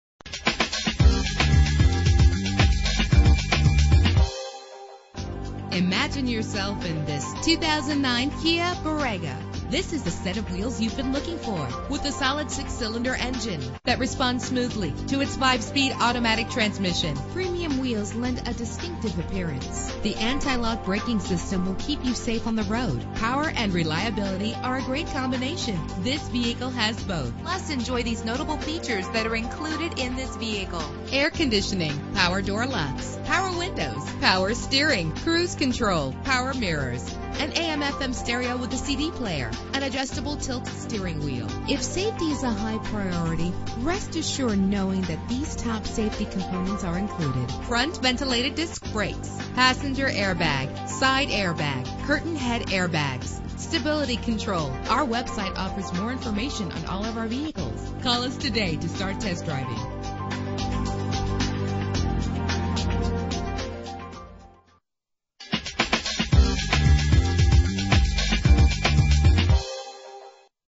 2009 Kia Borrego - Raleigh NC
Year: 2009
Make: Kia
Model: Borrego
Trim: LX
Engine: 3.8 liter 6 cylinder 24 valve
Transmission: 5-Speed Automatic
Color: Gray
Mileage: 33740
Address:
9209 Glenwood Ave